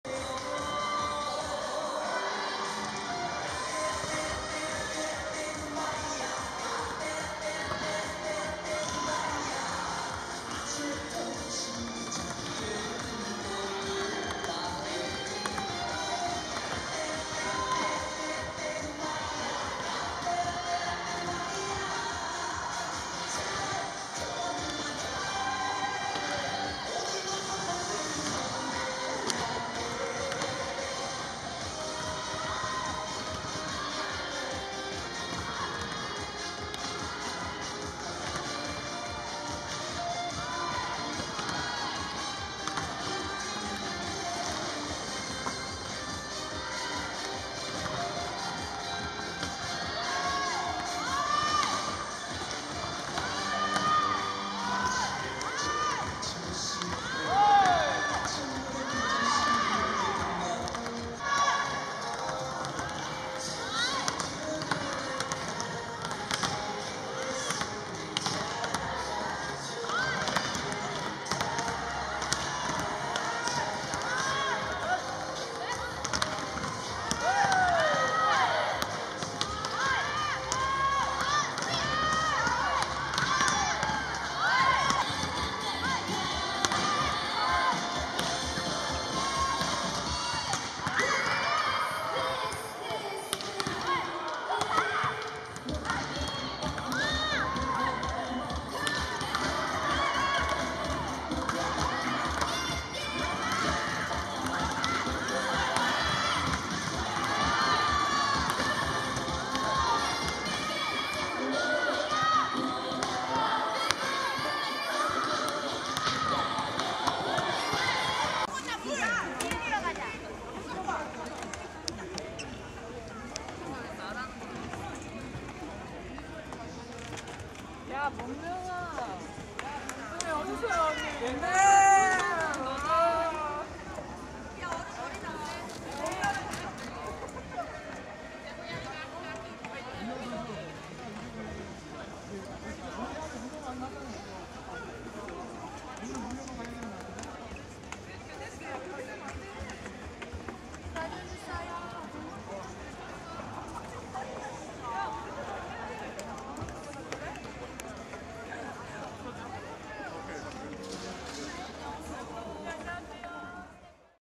'6연승 질주' GS칼텍스 이끄는 '쏘캡' 이소영의 리더십
[엑스포츠뉴스 장충, 박지영 기자] 12일 오후 서울 장충체육관에서 열린 '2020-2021 도드람 V리그' 여자부 GS칼텍스와 IBK기업은행의 경기, GS칼텍스가 세트 스코어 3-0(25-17, 25-17, 25-8)으로 승리하며 6연승을 질주, 다시 1위에 올랐다.

GS칼텍스를 이끄는 주장 이소영의 경기 전·후 모습을 영상으로 담았다.